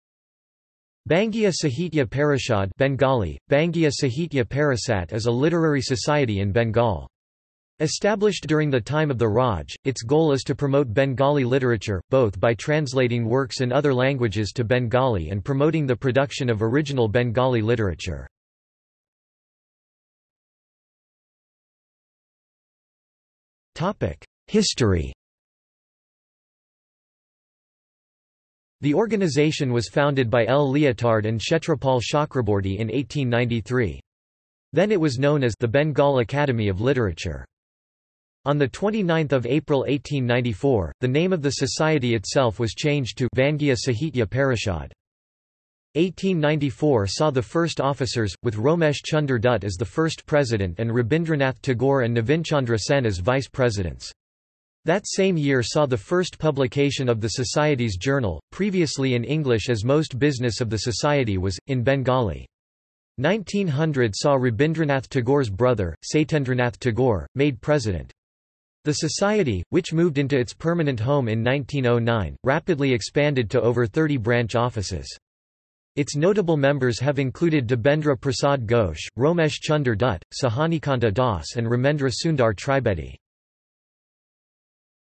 Bangiya Sahitya Parishad | Wikipedia audio article
This is an audio version of the Wikipedia Article:
Bangiya Sahitya Parishad

00:00:20 undefined

- Socrates

SUMMARY
Bangiya Sahitya Parishad (Bengali: বঙ্গীয় সাহিত্য পরিষৎ) is a literary society in Bengal. Established during the time of the Raj, its goal is to promote Bengali literature, both by translating works in other languages to Bengali and promoting the production of original Bengali literature.